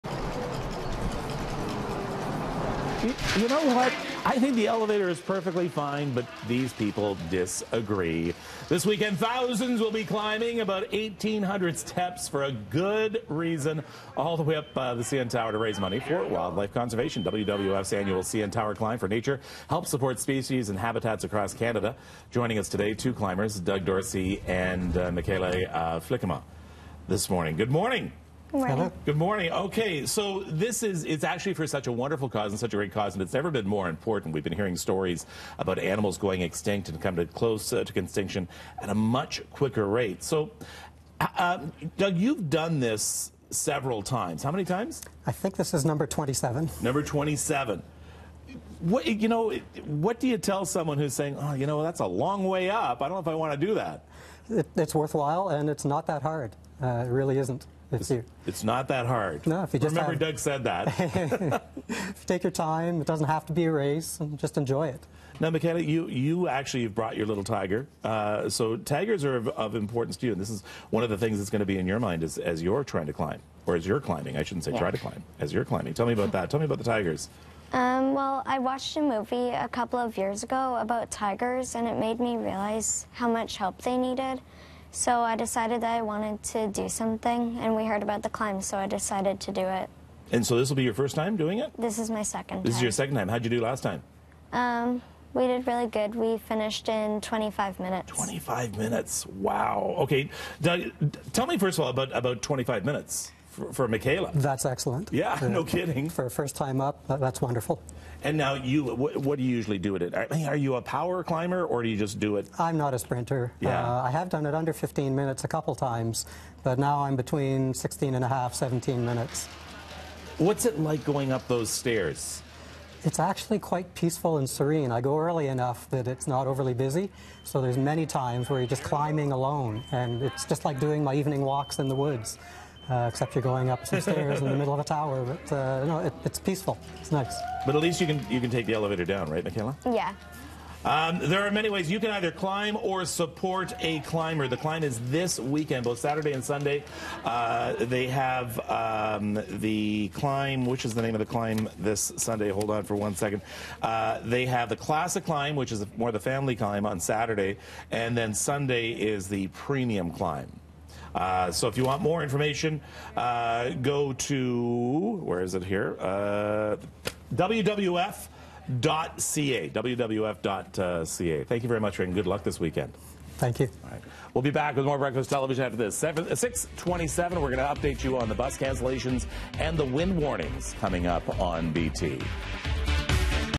WWF’s Annual CN Tower Climb for Nature
CN Tower Climbers Doug Dorsey and Mikayla Flikkema share details on this year's WWF CN Tower Climb for Nature, and it's positive impact on our wildlife.